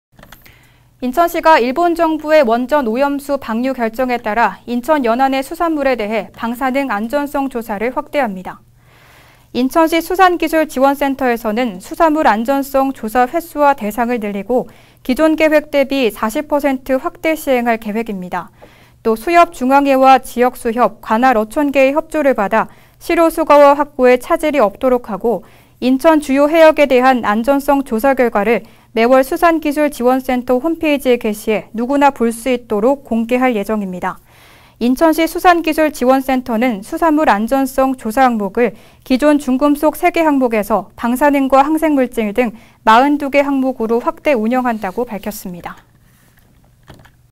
[NIB 뉴스] 인천시, 연안해역 수산물 방사능 안전성 조사 확대
인천시가 일본 정부의 원전 오염수 방류 결정에 따라 인천 연안의
수산물에 대해 방사능 안전성 조사를 확대합니다.

인천시 수산기술지원센터에서는 수산물 안전성 조사 횟수와 대상을
늘리고, 기존 계획 대비 40% 확대 시행할 계획입니다.

또 수협중앙회와 지역 수협, 관할 어촌계의 협조를 받아 시료 수거와 확보에 차질이 없도록 하고, 인천 주요 해역에 대한 안전성 조사 결과를 매월 수산기술지원센터 홈페이지에 게시해 누구나 볼 수 있도록 공개할 예정입니다.

인천시 수산기술지원센터는 수산물 안전성 조사 항목을 기존 중금속 3개 항목에서 방사능과 항생물질 등 42개 항목으로 확대 운영한다고 밝혔습니다.